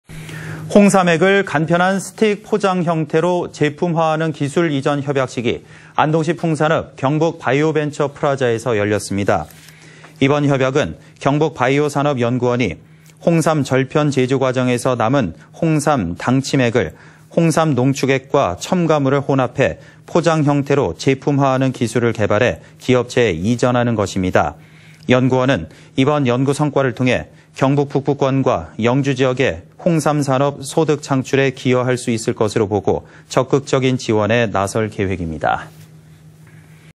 [안동MBC뉴스]홍삼식품개발 기술이전 협약
2016/07/07 09:55:31    작성자 : 조동진

홍삼액을 간편한 스틱 포장 형태로
제품화하는 기술이전 협약식이
안동시 풍산읍 경북바이오벤처프라자에서
열렸습니다.
◀ＥＮＤ▶

이번 협약은 경북바이오산업연구원이
홍삼 절편 제조과정에서 남은 홍삼 당침액을
홍삼농축액과 첨가물을 혼합해 포장형태로
제품화하는 기술을 개발해 기업체에 이전하는
것입니다.

연구원은 이번 연구성과를 통해
경북북부권과 영주지역의 홍삼산업 소득창출에
기여할 수 있을 것으로 보고 적극적인 지원에
나설 계획입니다.